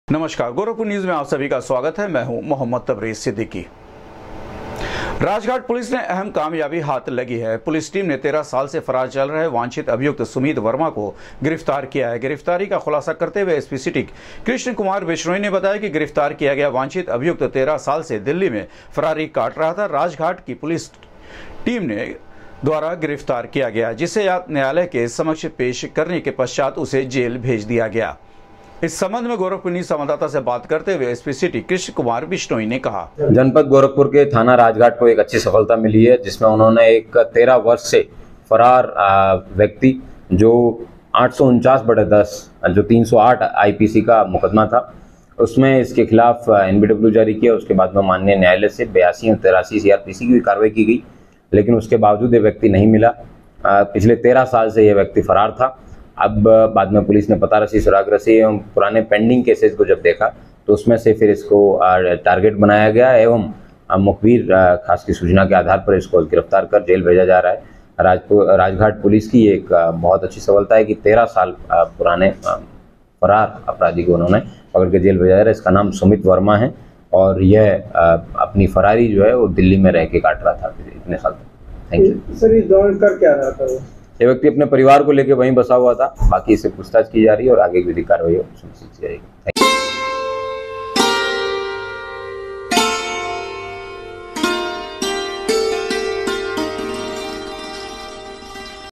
राजघाट पुलिस को अहम कामयाबी लगी हाथ, 13 साल से फरार चल रहे वांछित अभियुक्त सुमित वर्मा को किया
राजघाट पुलिस को अहम कामयाबी लगी हाथ, 13 साल से फरार चल रहे वांछित अभियुक्त सुमित वर्मा को किया गिरफ्तार, एसपी सिटी ने प्रेस वार्ता कर दी जानकारी